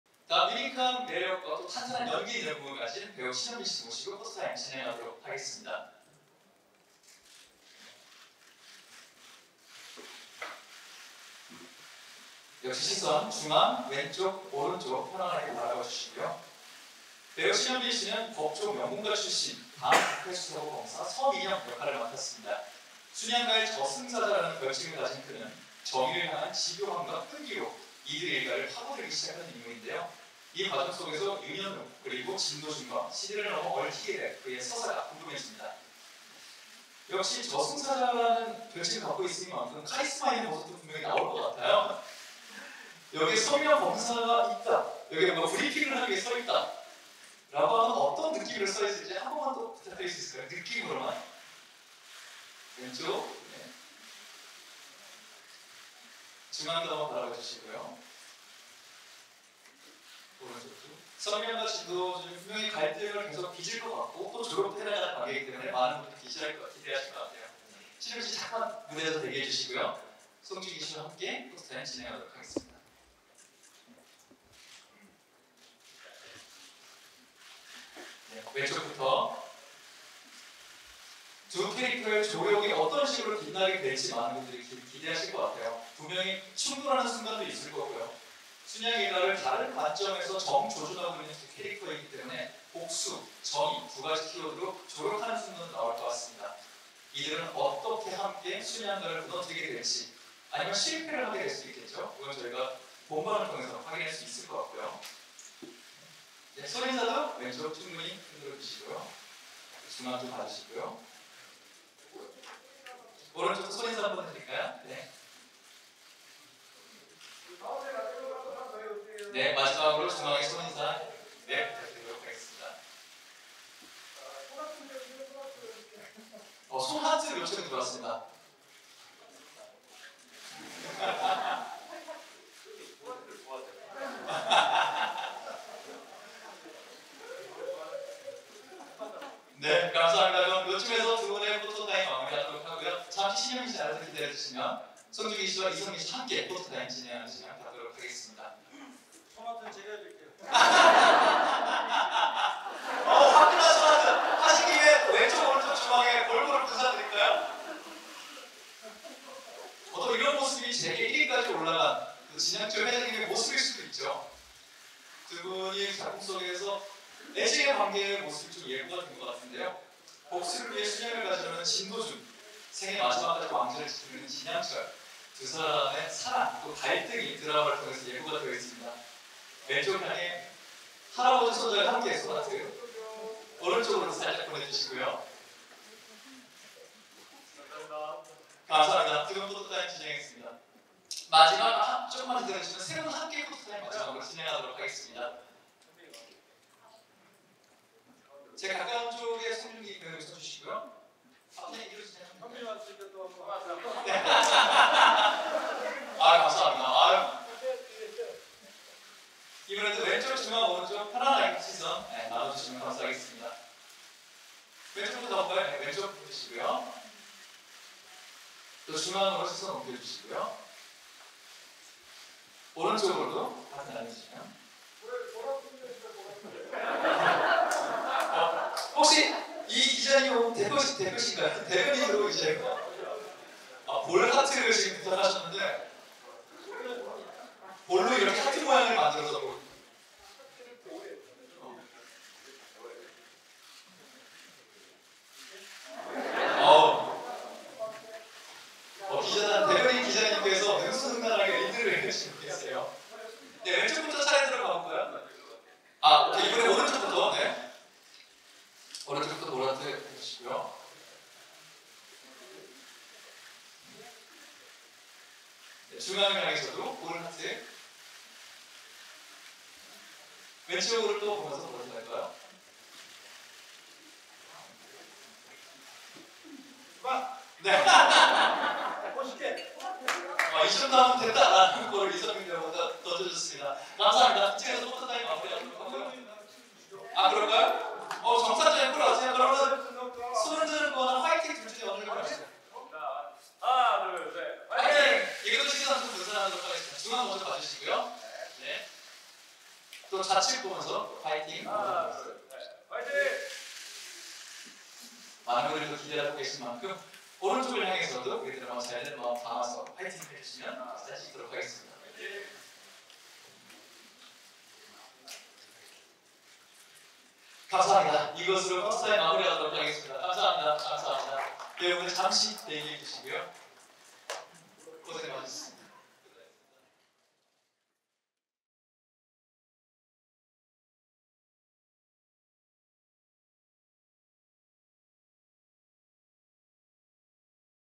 [4K] '재벌집 막내 아들' 제작발표회 신현빈&단체 포토타임｜'Reborn Rich' Press Conference Shin Hyun Been & Group Photo Time
'믿보배' 신현빈이 엘리트 검사로 돌아왔습니다😊

신현빈은 17일 열린 JTBC 금토일 드라마 '재벌집 막내아들' 제작발표회에 참여했는데요! 대기업을 집요하게 추적하는 검사와는 전혀 다른 청순한 미소를 이번 행사에서 선보였습니다😍

또한 송중기, 이성민과의 케미를 귀여운 하트로 증명했는데요! 재벌집 막내아들의 생생한 현장 소식, 지금 바로 만나보세요🙌

촬영·편집: 김정연 기자